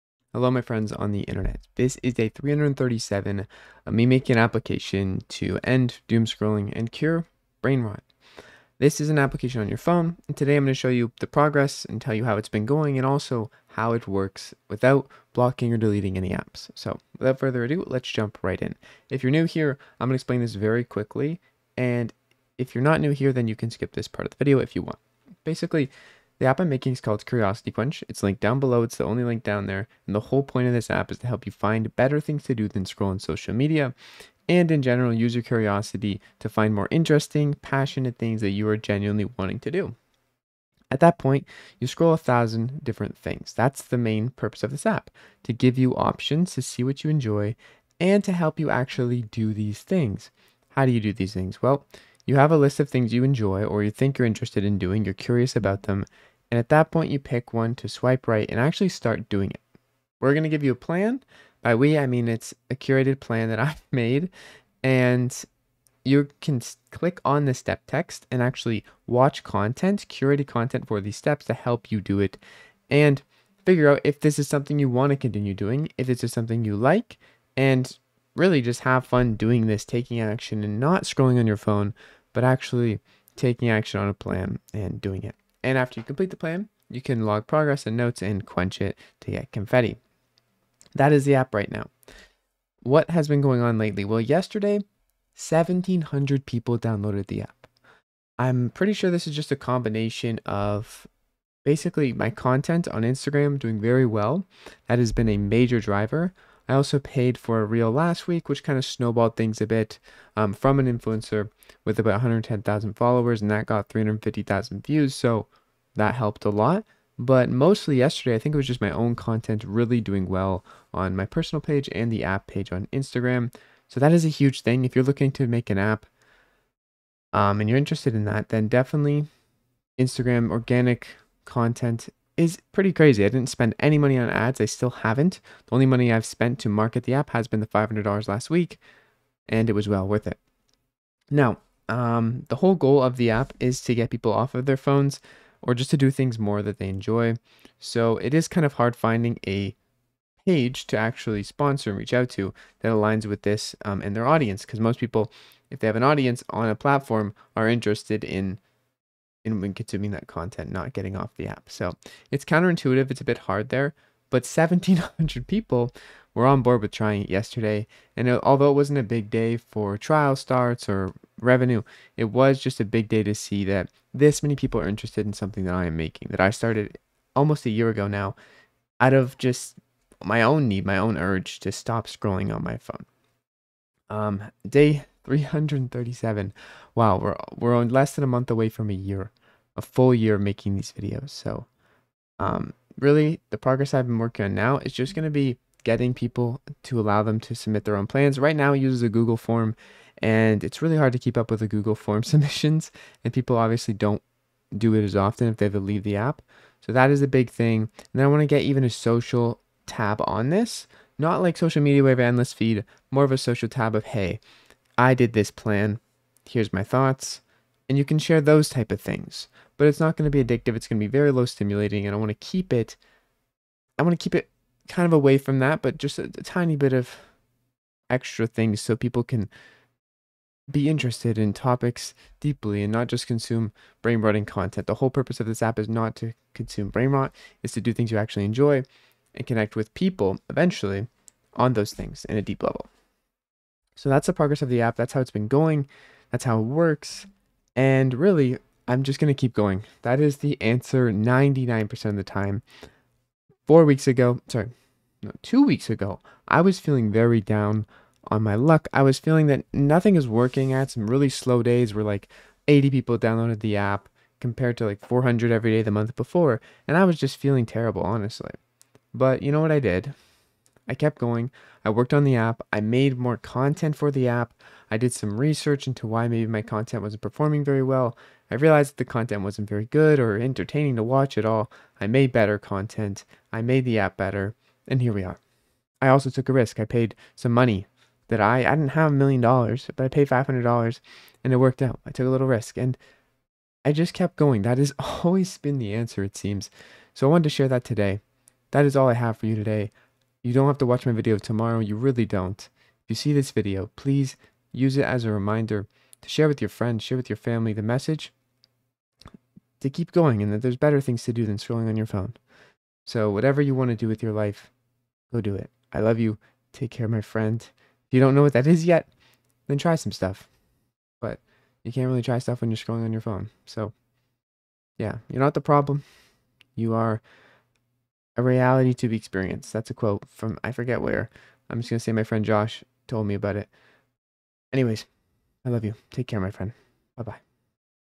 I'm making an app to END DOOMSCROLLING (day 337, build log)
Replace endless scrolling with purposeful action:
(the app im making called Curiosity Quench)